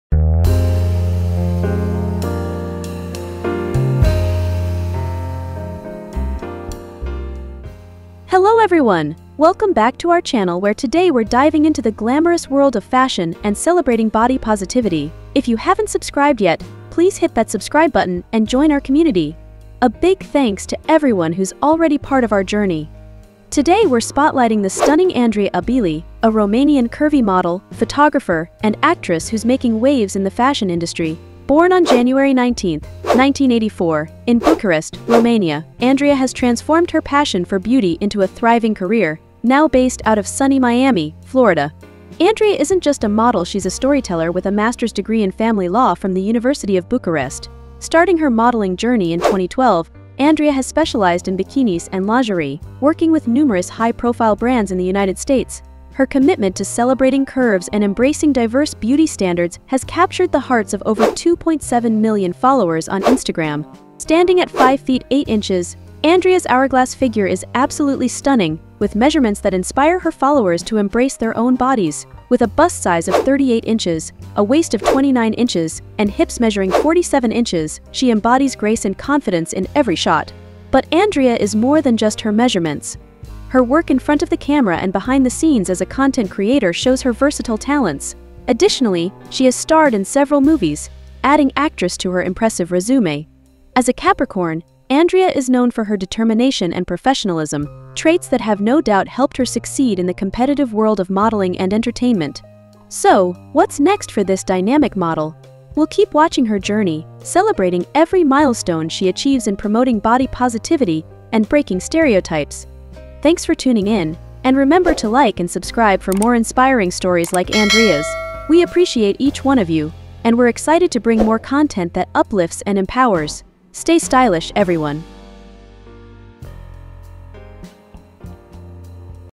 Andrea Abeli (Curvy Model) Wiki, Fashion, Height, Biography & More
Welcome back! Today, we're spotlighting Andrea Abeli, a Romanian Curvy Model known for her empowering presence in the fashion world. Born in Bucharest in 1984, Andrea now thrives in Miami, Florida.

With a degree in family law, she started modeling in 2012, specializing in bikinis and lingerie. Her Instagram boasts over 2.7 million followers, drawn to her stunning hourglass figure and captivating storytelling.

Standing at 5'8" with measurements of 38-29-47 inches, she exudes confidence and grace. Andrea's work extends beyond modeling; she's also a photographer, actress, and social media influencer.